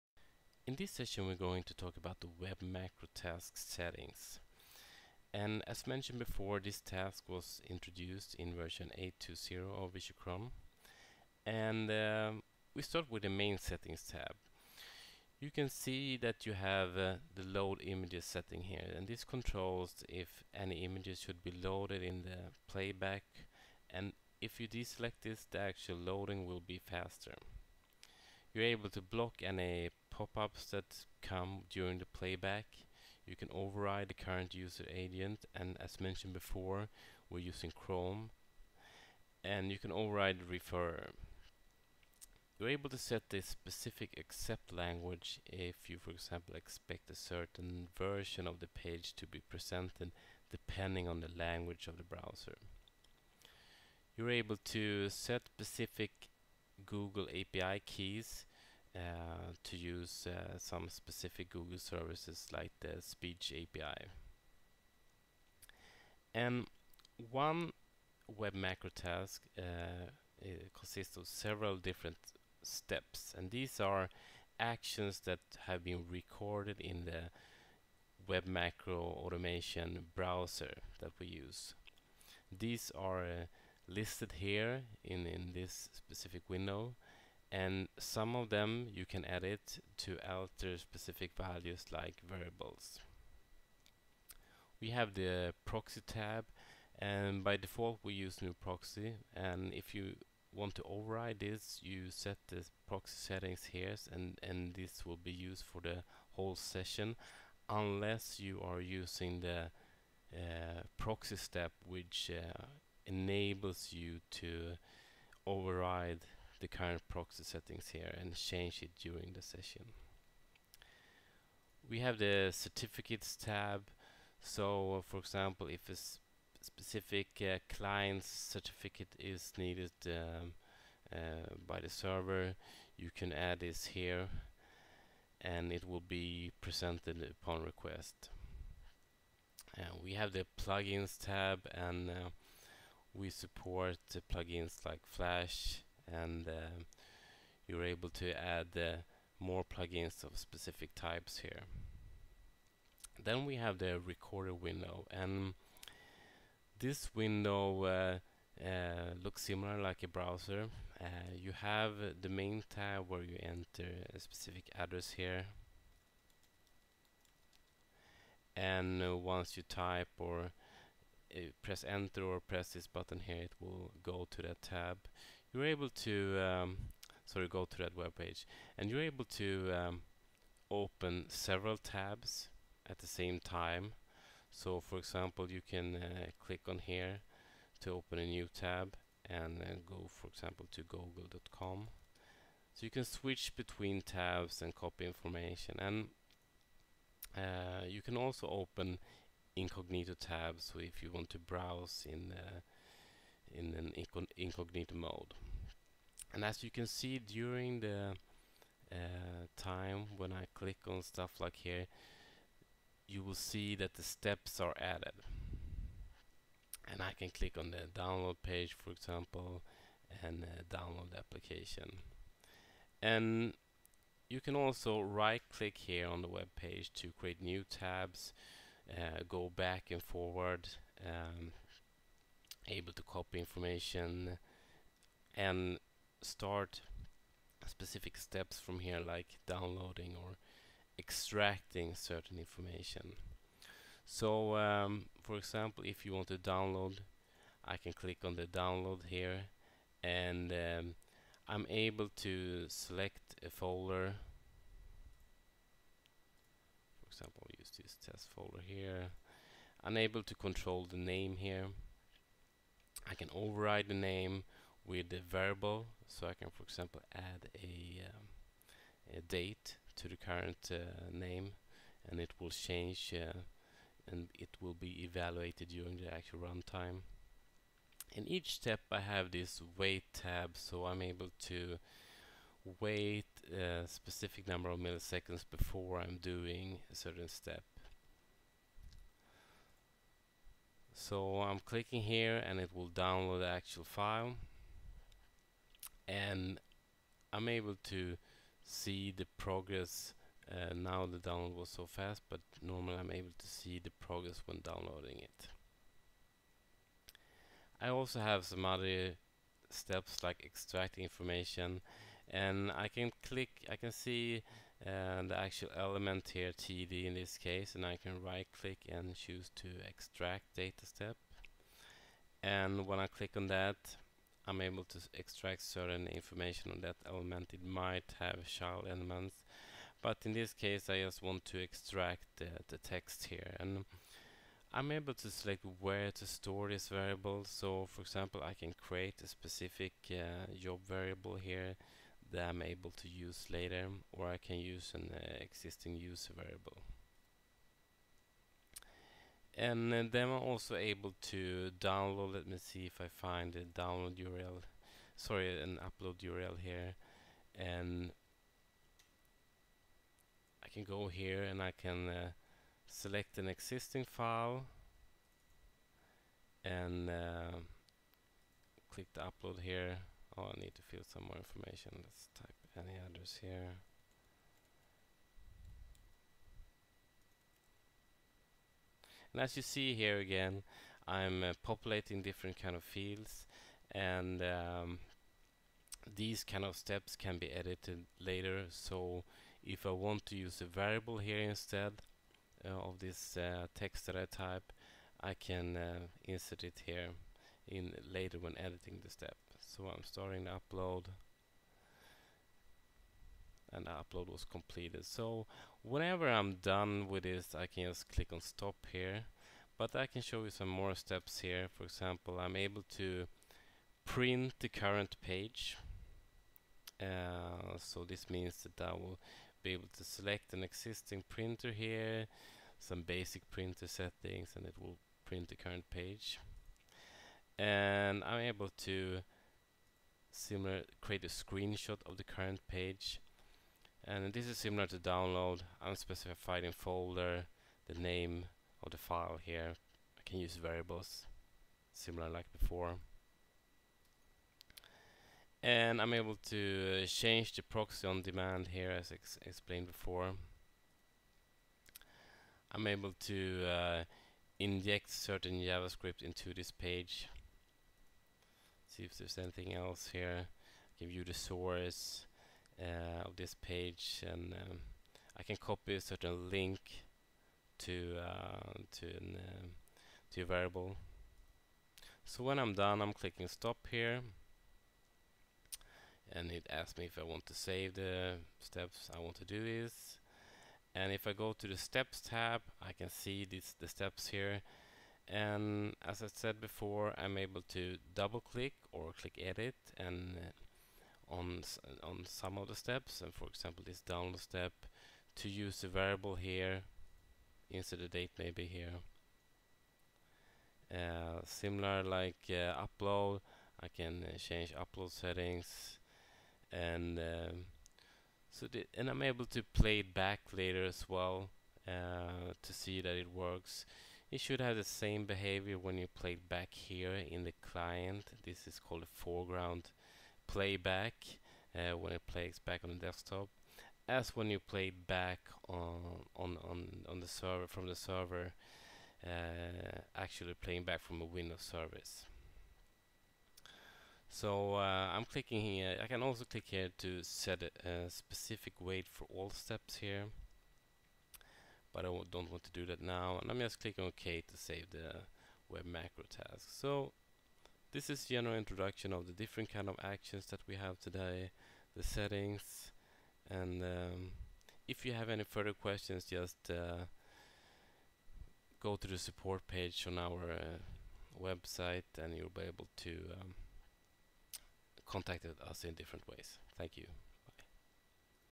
Web macro recorder - steps
This video is from the web macro recorder Task in VisualCron - task scheduler and automation tool for Windows.